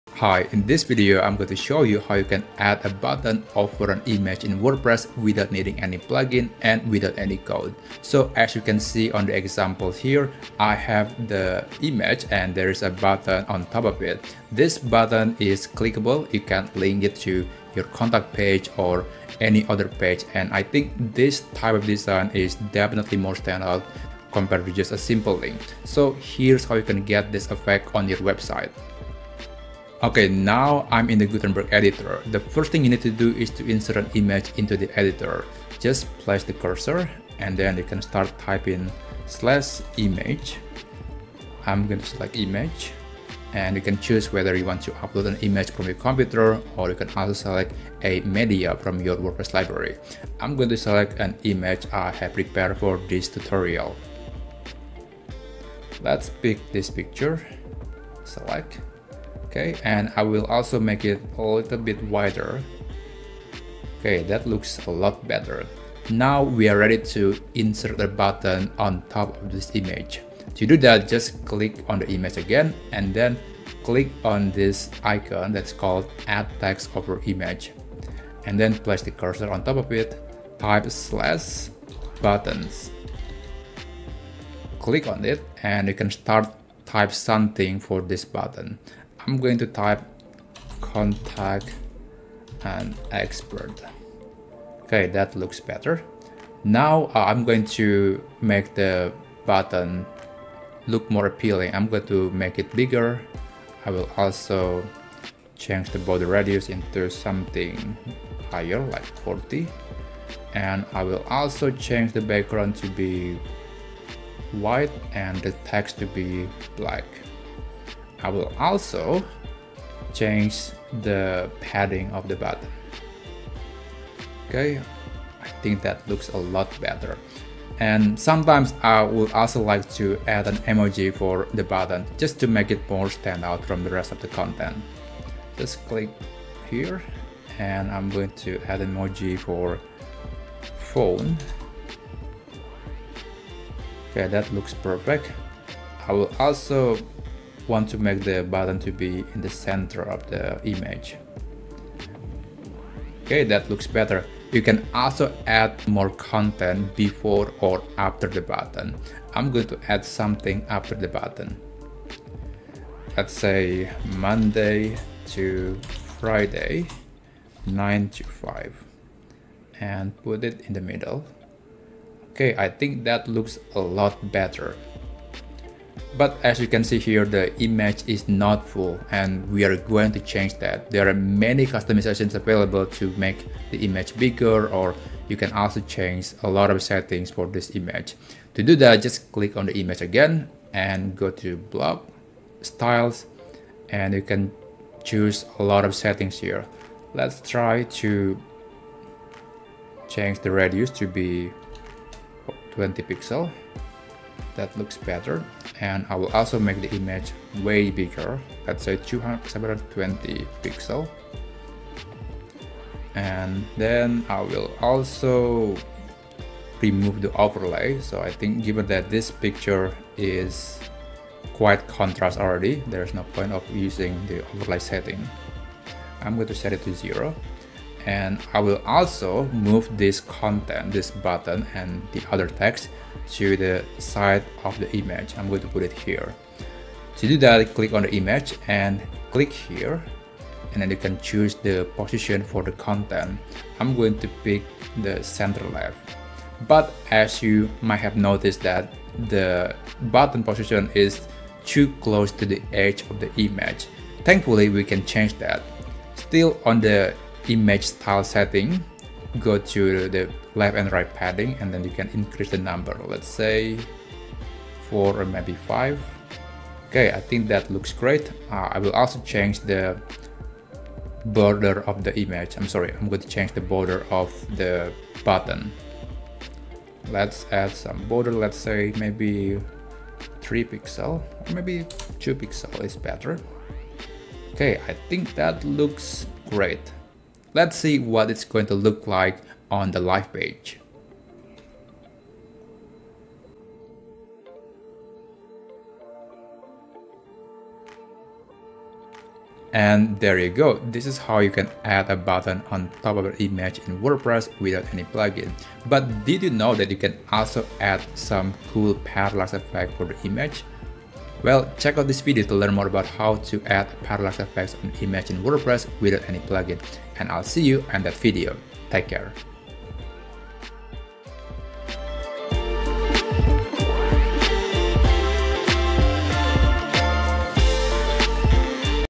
How to Add a Button on Image in WordPress, But Without CSS
In this tutorial, I will show you how to add a button over an image in WordPress without the need for any plugins. This method is perfect for those who want to customize their website and have full control over its appearance. By following these simple steps, you can easily create engaging buttons that draw attention to your content or call-to-action.

00:00 Intro
00:30 How to add button on image
04:47 Final result

Boom arm 💥